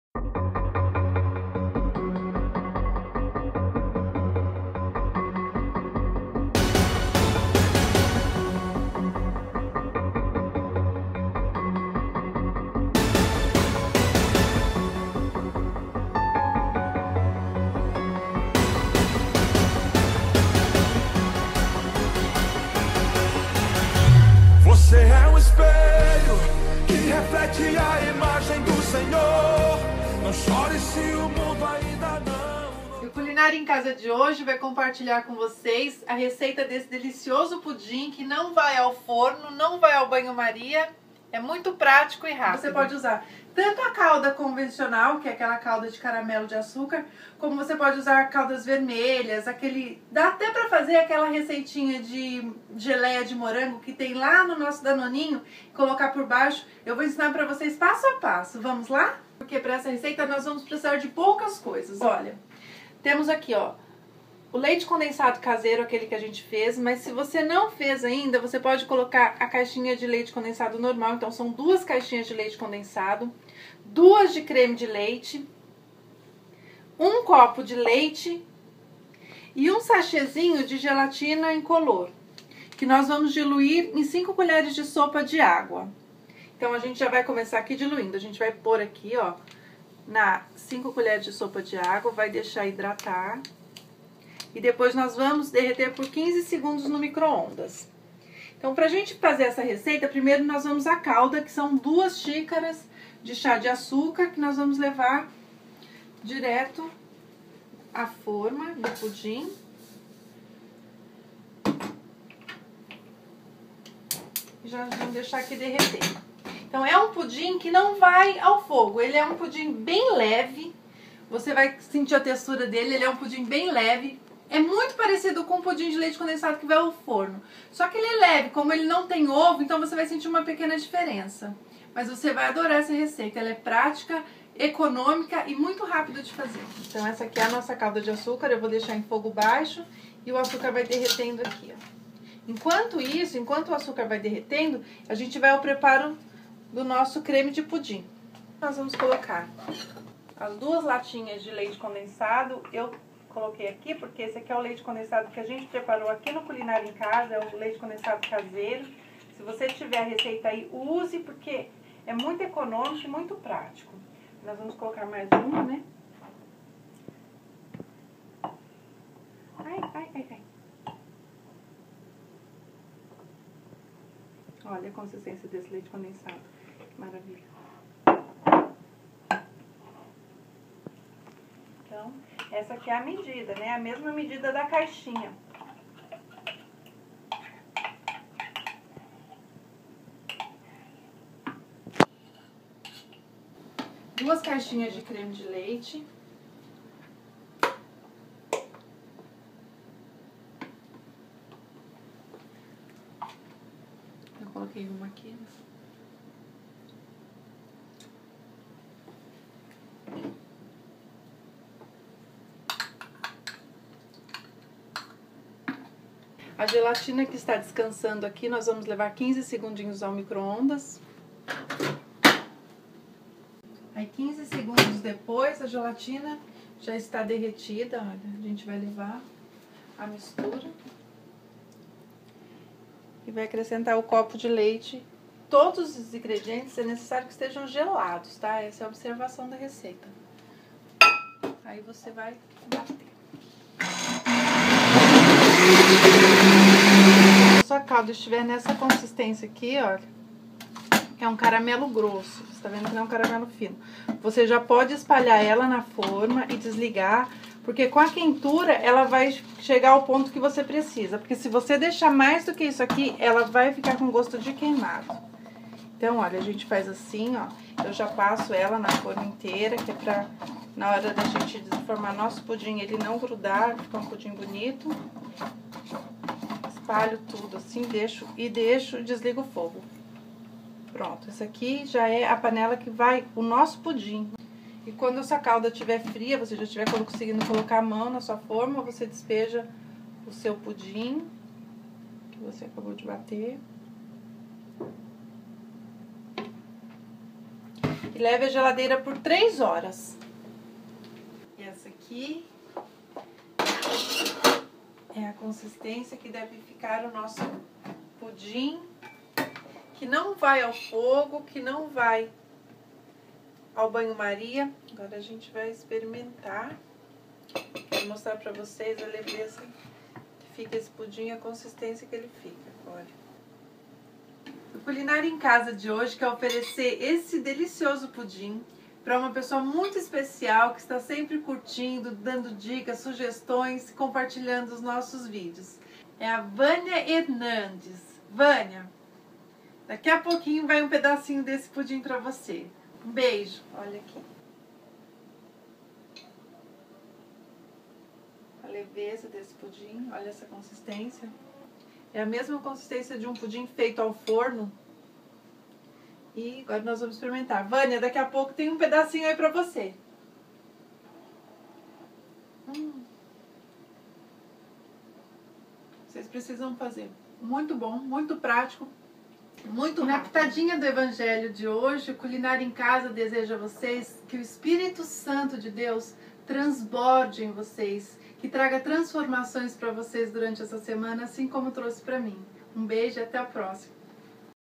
Pudim que não vai ao forno - Culinária em casa
Culinária em Casa -   pudim que não vai ao forno

INGREDIENTES

2 lata de leite condensado
2 caixinha de creme de leite ( previamente gelado)
1 copo  de leite
2  pacote de gelatina sem sabor
2 xicaras  de cha  de açúcar (para a calda)
Hidrate a gelatina em pó sem sabor e dissolva no leite
Num liquidificador coloque 2 lata de leite condensado, 2 caixinha de creme de leite e a gelatina já hidratada e dissolvida no leite e bata bem até formar uma mistura homogênea - reserve
Em uma fôrma de pudim coloque o açúcar e mexa bem até dissolver e formar uma calda, unte toda a forma
Coloque a mistura batida no liquidificador
Leve ao freezer por aproximadamente 1 hora,ou na geladeira por 3 horas  desenforme e sirva e depois, manter em geladeira
Contato Profissional:
QUER ME ENVIAR MIMOS?
CAIXA POSTAL: 79146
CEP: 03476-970
SÃO PAULO - SP
Luciana Mirihad *